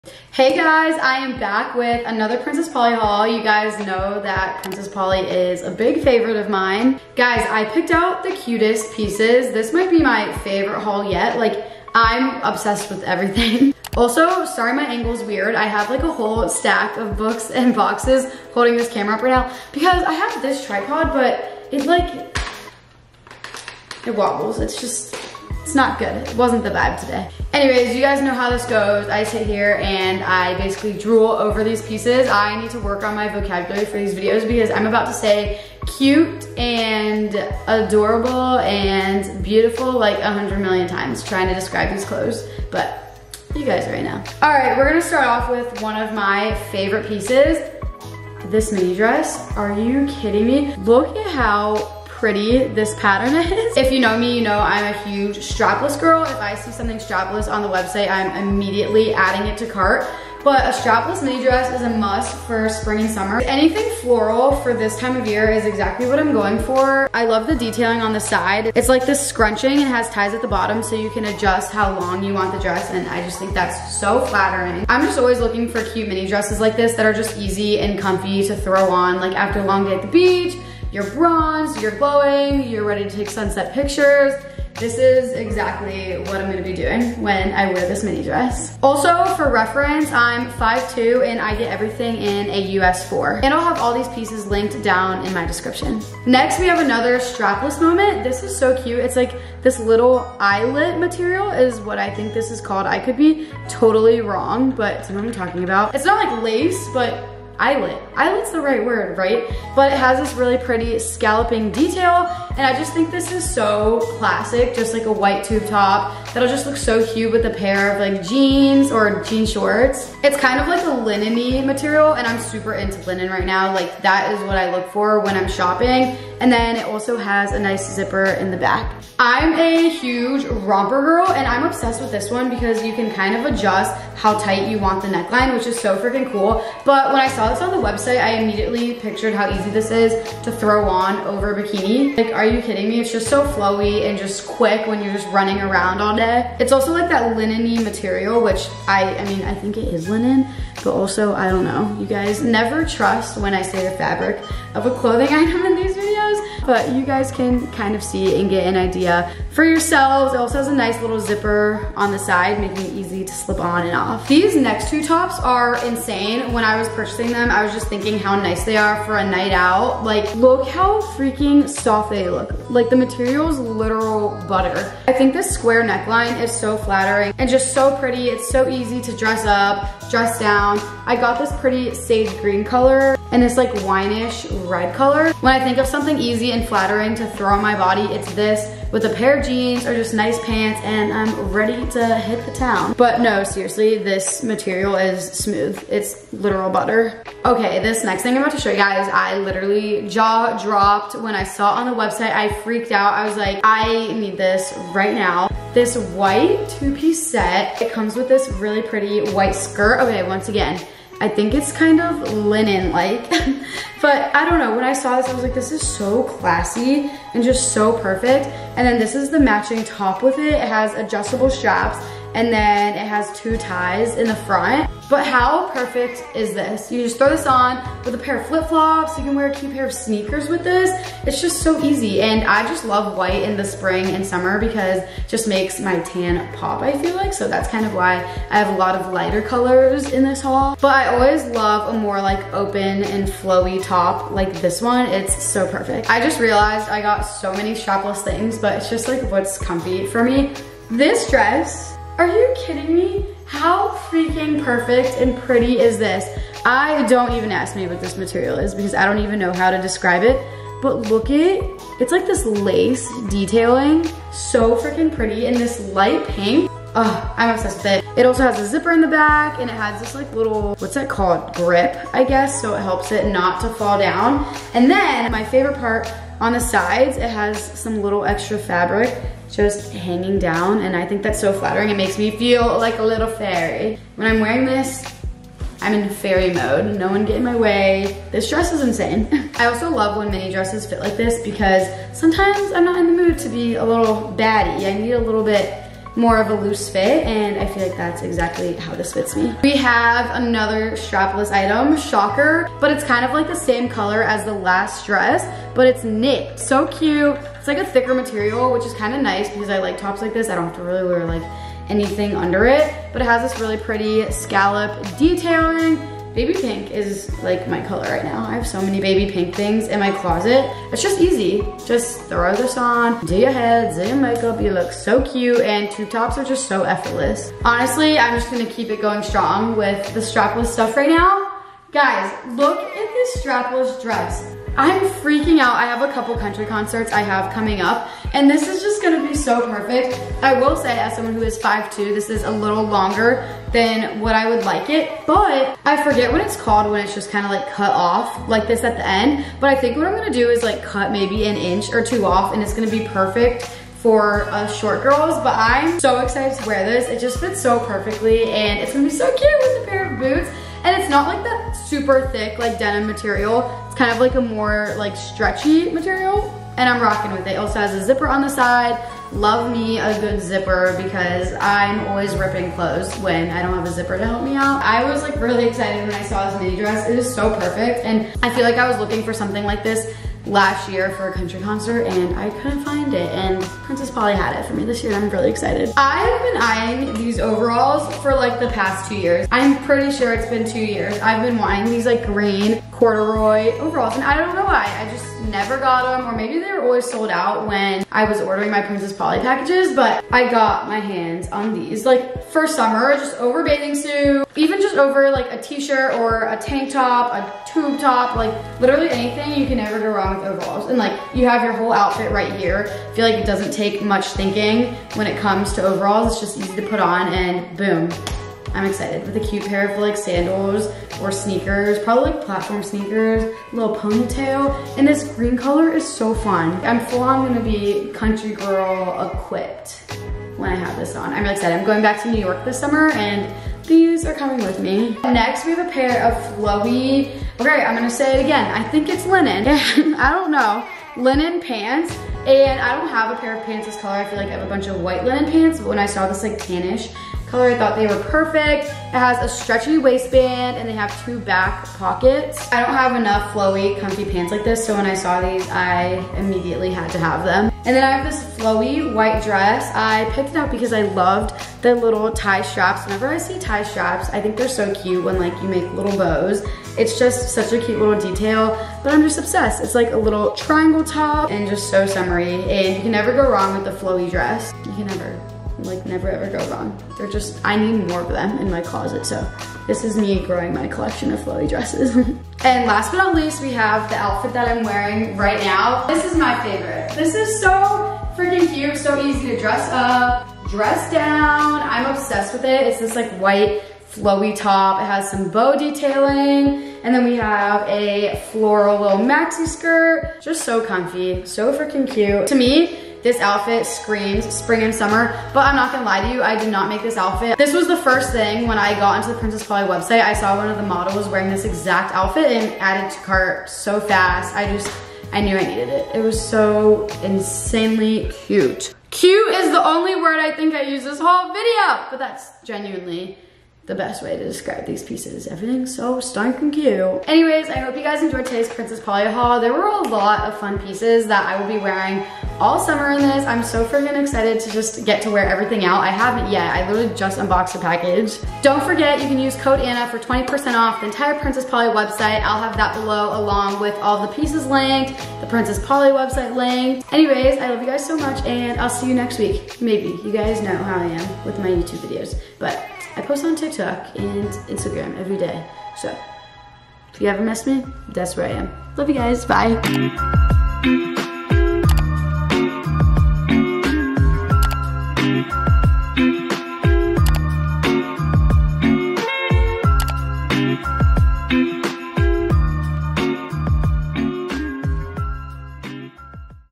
Spring Clothing Haul!
I hope you guys enjoyed the video! Princess Polly is one of my fav places to buy fun and cute pieces to spice up my closet:,)
-Free Standard Shipping (3-6 days) to the US on orders over $50 USD
-Princess Polly also offers AfterPay and ShopPay to all US customers:)
ILY GUYS!